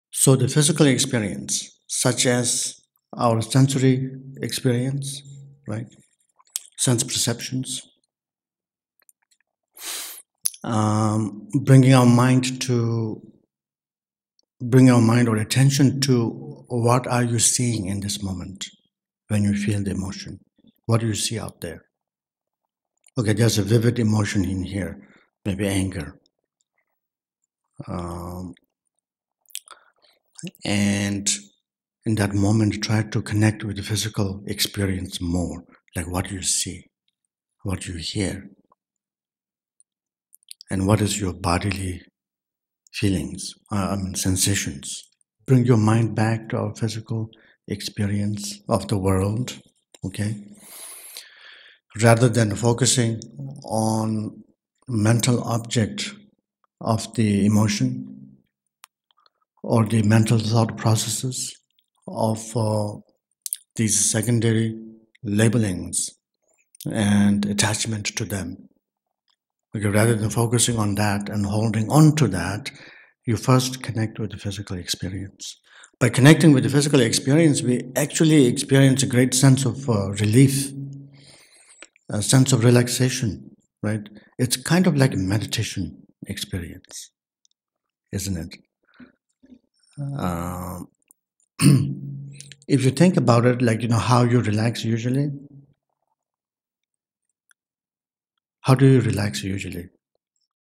EMOTIONAL RESCUE: CONNECTING BODY & MIND
Dzogchen Ponlop Rinpoche teaches how using sense perceptions and sensations is one way to connect and understand emotion.